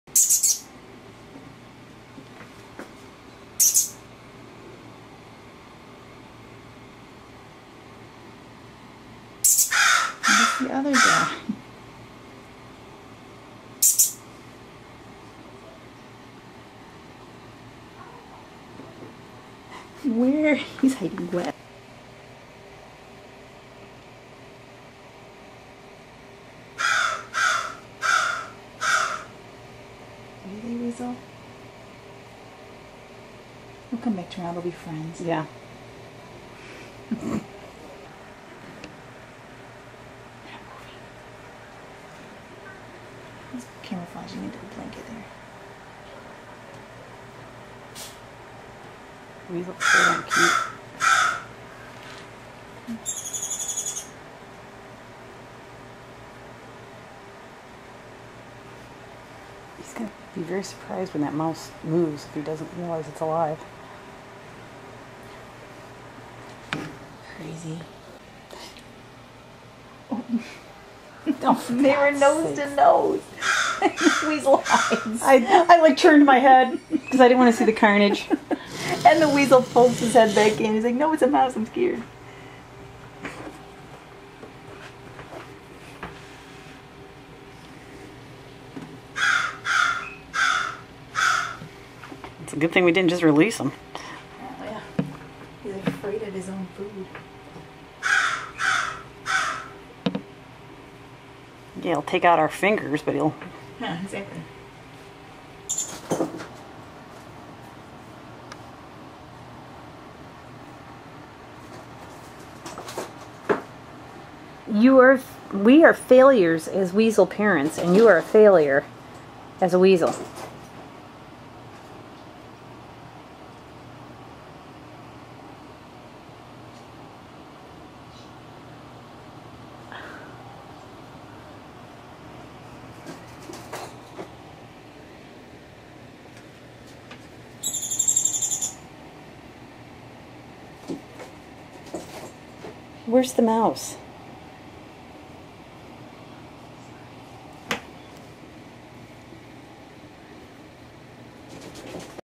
Least Weasel, No Mice Were Hurt Nature's Nursery
There is a reason we give predators live prey before they are released. It isn't our favorite part of the job we do, but it does no good to raise an animal only on dead prey if they are going to need to hunt once released. This weasel was vicious when "hunting" a dead mouse...but not so much the case with live! I have no doubt he will figure it out...but it certainly was not the case this first time having live prey. We are moving him to a larger cage with more natural substrate...maybe that will put him in the mood!